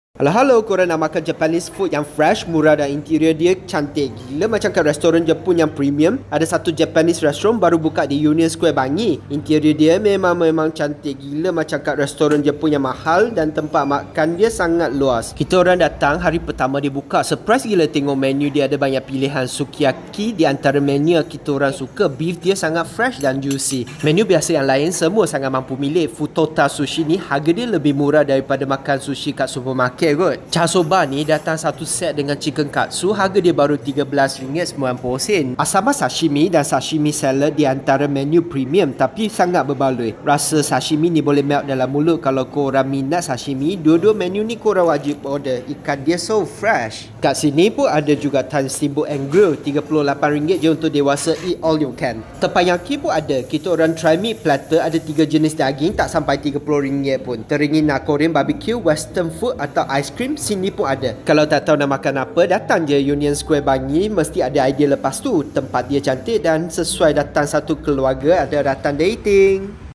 Restoran Jepun Mahal tapi Harga Murah??
Sukiyaki dan sushi lovers wajib singgah restoran Jepun terbaru di Bangi, harga biasa- biasa tapi makan kat suasana restoran mahal.